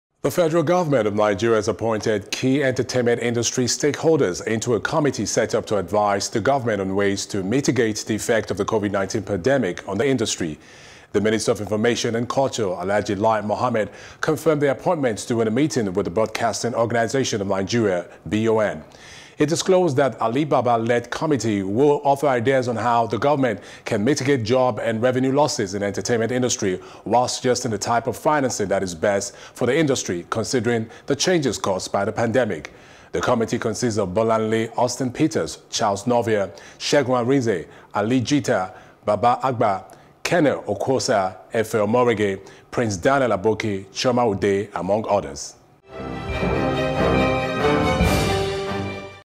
FG Appoints Ali Baba, Segun Arinze Others On COVID-19 Committee
The Federal Government of Nigeria has appointed key entertainment industry stakeholders into a committee set up to advise the government on ways to mitigate the effect of the Covid-19 pandemic on the industry.